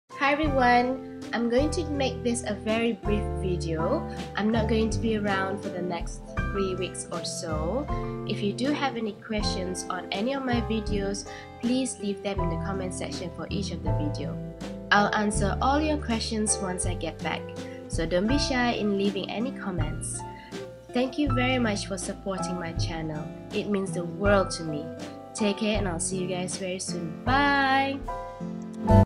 A short video on my leave of absence :)
Hi everyone! Like the video says, leave your questions on each of the video's comment sections. I'll get back to you as soon as I come back. You know I'll always reply back. Hahaha.. Thank you for supporting my channel! :)